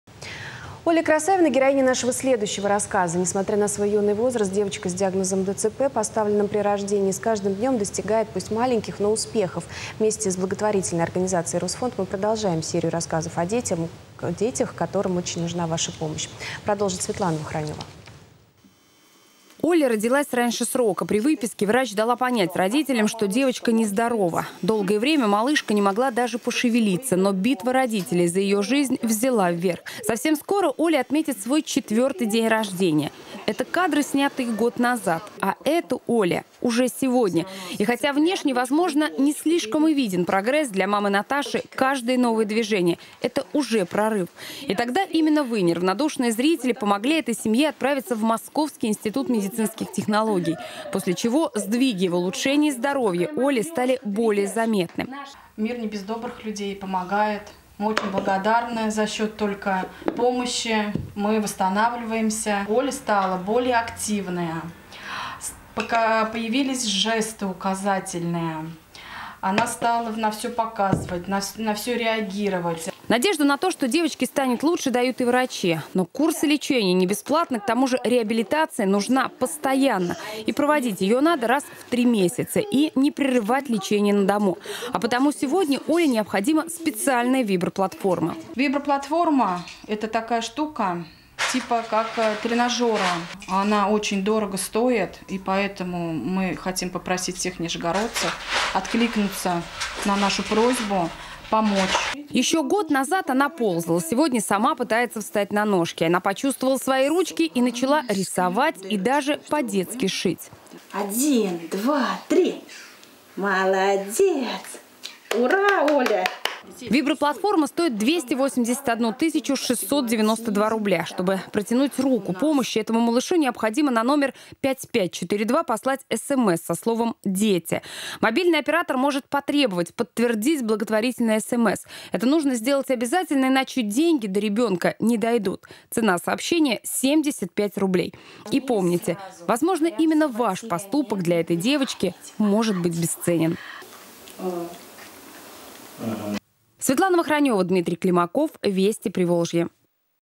Оля Красавина
Оля Красавина, 3 года, детский церебральный паралич, требуется виброплатформа. 41 311 руб.